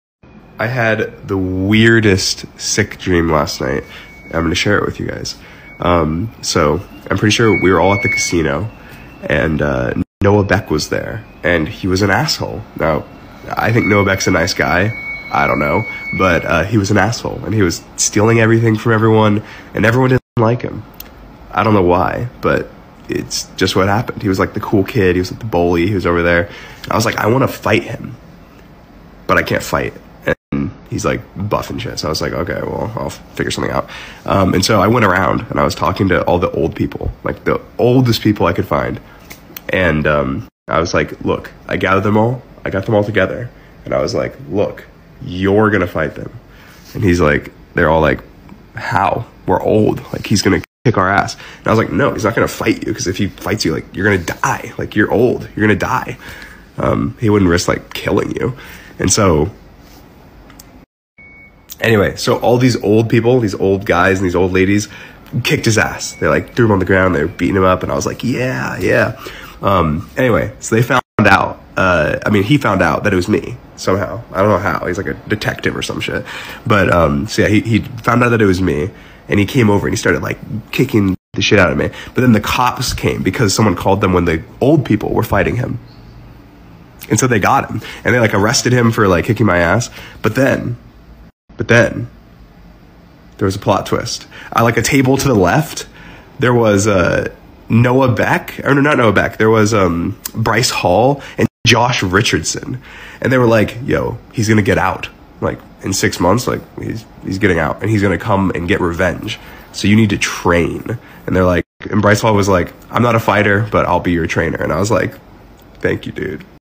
Dream had a dream about Noah Beck
Dream dreamt(lol) about fighting  noah beck and bryce hall and josh richards were there ?